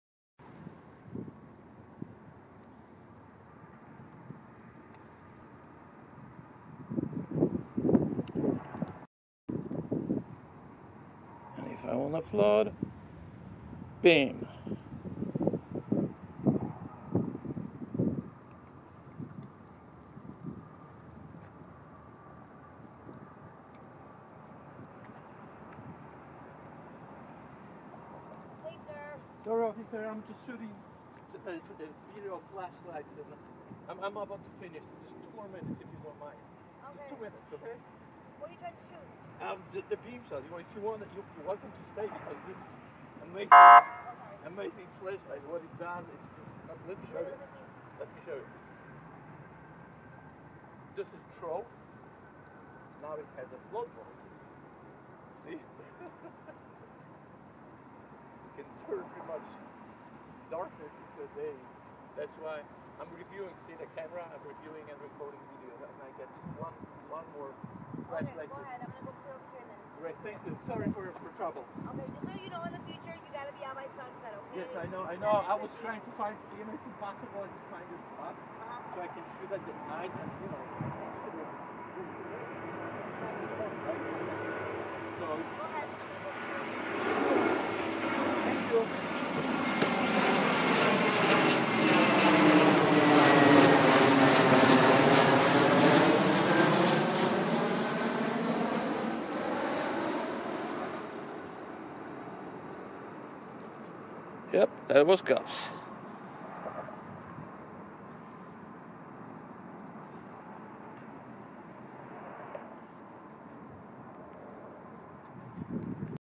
Full video of encounter with Cops while shooting beam shot video of Fenix LR40R ....
Per your request here is uncut video .